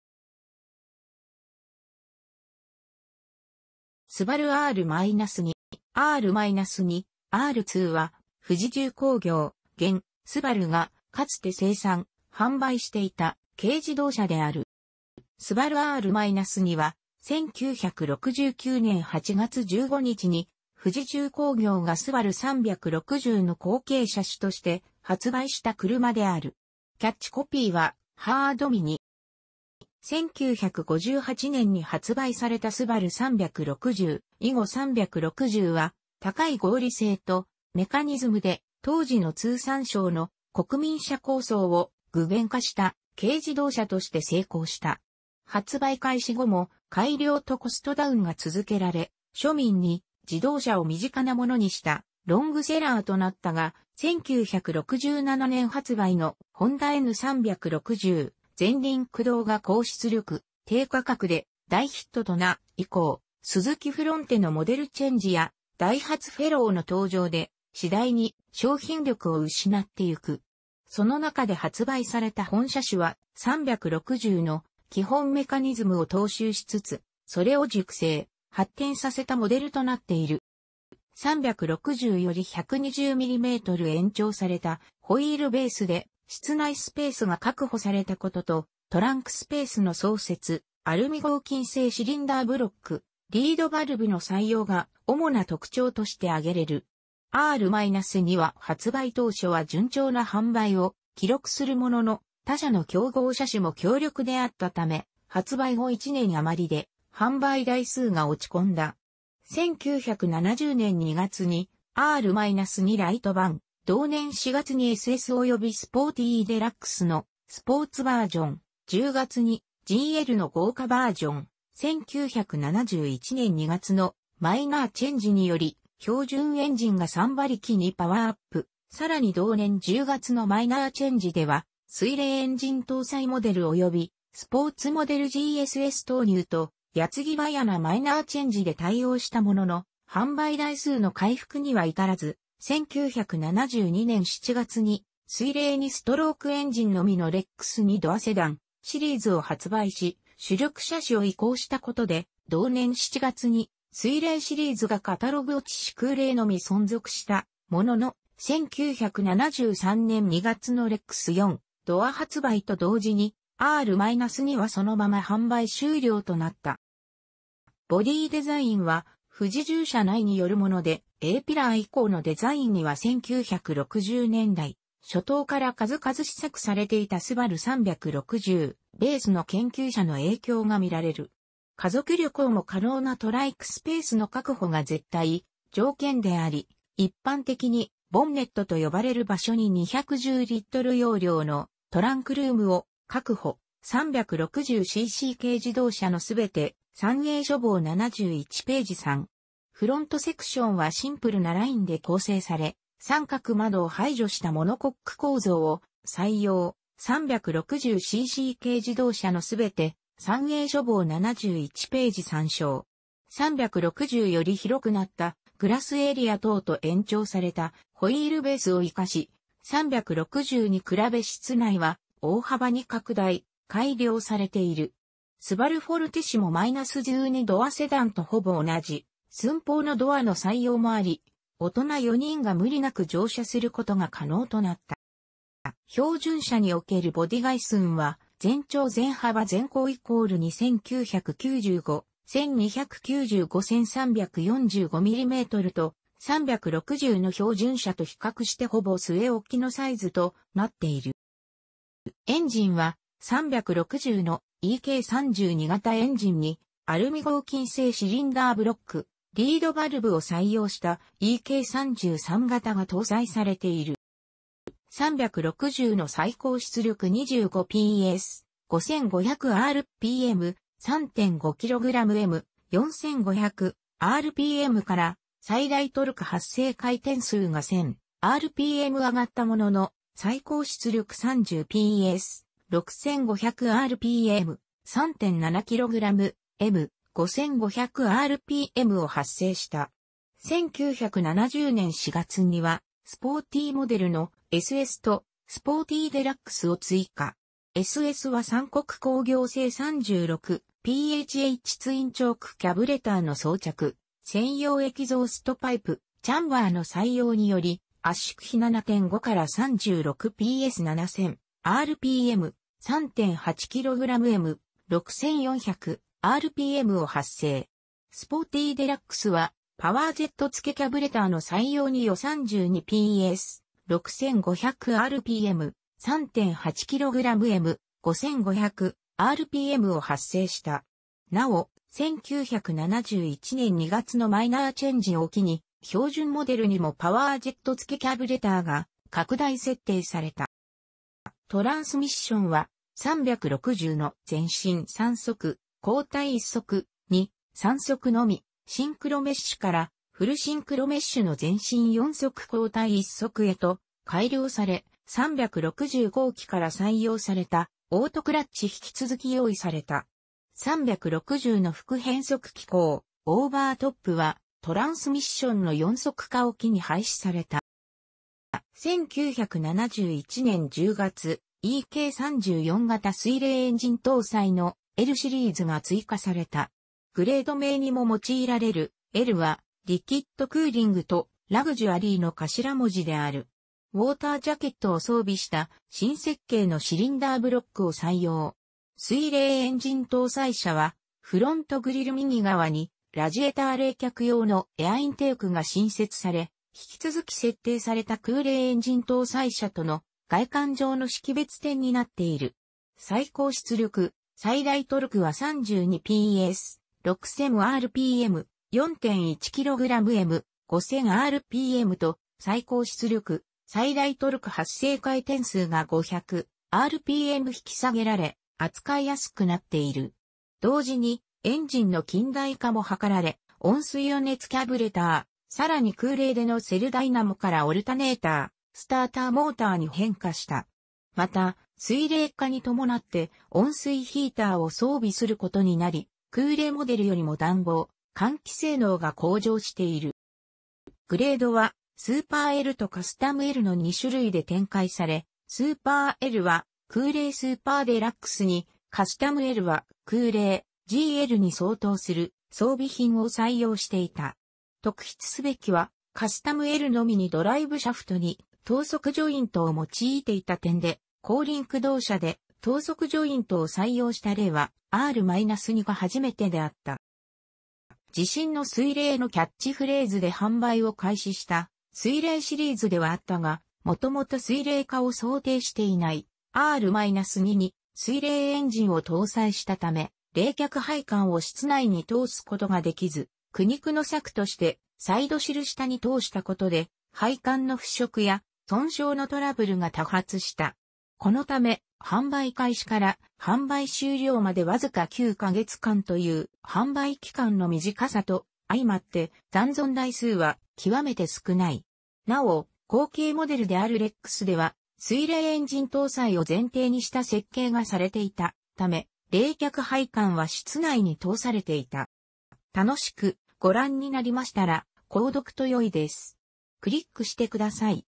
スバル・R-2
R-2（アールツー）は、富士重工業（現・SUBARU）がかつて生産・販売していた軽自動車である。

スバル・R-2は、1969年8月15日に富士重工業がスバル・360の後継車種として発売した車である。キャッチコピーは「ハードミニ」。

1958年に発売されたスバル・360（以後360）は、高い合理性とメカニズムで、当時の通産省の「国民車構想」を具現化した軽自動車として成功した。発売開始後も改良とコストダウンが続けられ、庶民に自動車を身近なものにしたロングセラーとなったが、1967年発売のホンダ・N360（前輪駆動）が高出力・低価格で大ヒットとなった以降、スズキ・フロンテのモデルチェンジやダイハツ・フェローの登場で、次第に商品力を失っていく。その中で発売された本車種は、360の基本メカニズムを踏襲しつつ、それを熟成、発展させたモデルとなっている。

360より120mm延長されたホイールベースで室内スペースが確保されたことと、トランクスペースの創設、アルミ合金製シリンダーブロック、リードバルブの採用が主な特徴として挙げられる。R-2は発売当初は順調な販売を記録するものの、他社の競合車種も強力であったため、発売後1年余りで販売台数が落ち込んだ。1970年2月に、R-2ライトバン、同年4月にSSおよびスポーティーデラックスのスポーツバージョン、10月にGLの豪華バージョン、1971年2月のマイナーチェンジにより標準エンジンが32馬力にパワーアップ、さらに同年10月のマイナーチェンジでは水冷エンジン搭載モデルおよびスポーツモデルGSS投入と、矢継ぎ早なマイナーチェンジで対応したものの、販売台数の回復には至らず、1972年7月に水冷2ストロークエンジンのみのレックス2ドアセダン・シリーズを発売し、主力車種を移行したことで同年7月に水冷シリーズがカタログ落ちして空冷のみ存続したものの、1973年2月のレックス4ドア発売と同時にR-2はそのまま販売終了となった。

ボディー・デザインは富士重社内によるもので、Aピラー以降のデザインには1960年代初頭から数々試作されていたスバル・360ベースの研究車の影響が見られる。「家族旅行も可能なトランクスペース」の確保が絶対条件であり、一般的にボンネットと呼ばれる場所に210L容量のトランクルームを確保（『360cc軽自動車のすべて』三栄書房71頁参照）。フロントセクションはシンプルなラインで構成され、三角窓を排除したモノコック構造を採用（『360cc軽自動車のすべて』三栄書房　71頁参照）。360より広くなったグラスエリア等と延長されたホイールベースを生かし、360に比べ室内は大幅に拡大、改良されている。スバル・ff-12ドアセダンとほぼ同じ寸法のドアの採用もあり、大人4人が無理なく乗車することが可能となった。

標準車におけるボディ外寸は、全長×全幅×全高＝2,995×1,295×1,345mmと、360の標準車と比較してほぼ据え置きのサイズとなっている。

エンジンは、360の「EK32型」エンジンに、アルミ合金製シリンダーブロック、リードバルブを採用した「EK33型」が搭載されている。

360の最高出力25PS/5,500rpm、3.5kg-m/4,500rpmから、最大トルク発生回転数が1,000rpm上がったものの、最高出力30PS/6500rpm、3.7kg-m/5500rpmを発生した。1970年4月には、スポーティーモデルの「SS」と「スポーティーデラックス」を追加。「SS」は三国工業製36PHHツインチョーク・キャブレターの装着、専用エキゾーストパイプ、チャンバーの採用により、圧縮比7.5から36PS7,000rpm、3.8kg-m/6,400rpmを発生。「スポーティーデラックス」はパワージェット付キャブレターの採用により32PS/6,500rpm、3.8kg-m/5,500rpmを発生した。なお、1971年2月のマイナーチェンジを機に、標準モデルにもパワージェット付キャブレターが拡大設定された。

トランスミッションは、360の前進3速+後退1速、2,3速のみシンクロメッシュから、フルシンクロメッシュの前進4速+後退1速へと改良され、360後期から採用された「オートクラッチ」も引き続き用意された。360の副変速機構「オーバートップ」はトランスミッションの4速化を機に廃止された。

1971年10月、「EK34型」水冷エンジン搭載の「L」シリーズが追加さ...